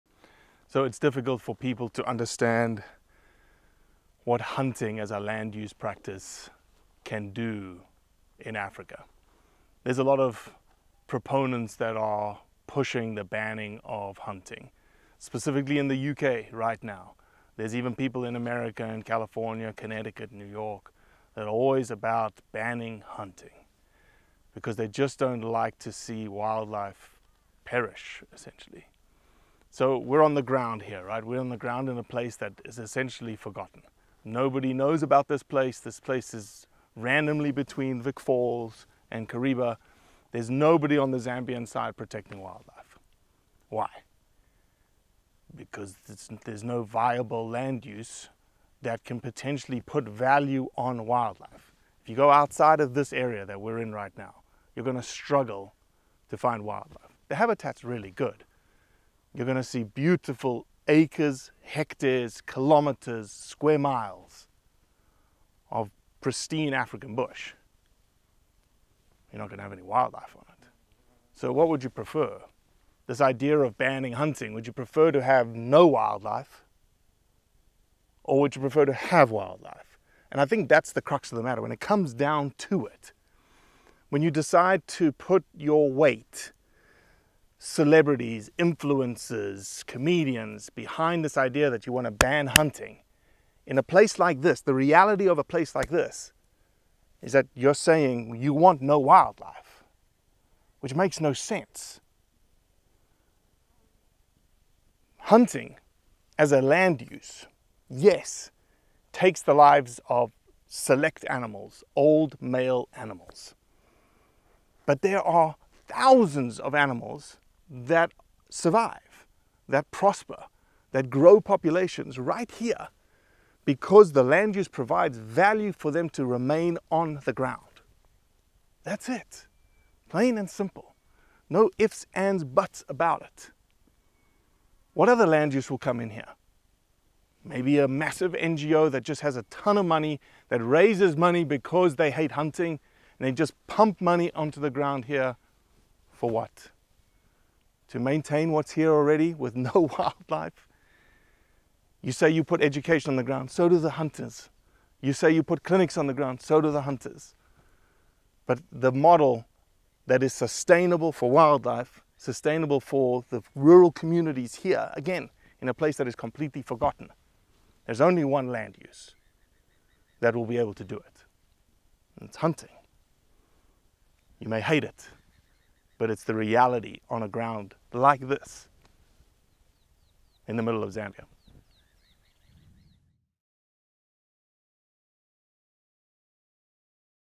Ban hunting to save wildlife or use hunting to save it?
The paradox is a real one - how can hunting purport to love animals yet kill them at the same time? It's simple, because it then creates a hundred fold or a thousand fold more animals. Thats it. All of these bans around the world - the Animal Abroad Bill in the UK and the NY Senate Bill right now (Assembly Bill 584 and 518) are intended to indirectly ban hunting. From physical observations on the ground in many places in Africa - there are wildlife existing in these areas because hunting is protecting them and allowing them to be nutured to increase in population. In areas right next door to these places where there is no-one protecting wildlife there is no wildlife. So the simple question is this: do you want more willdife or do you want less wildlife? So here's the truth ...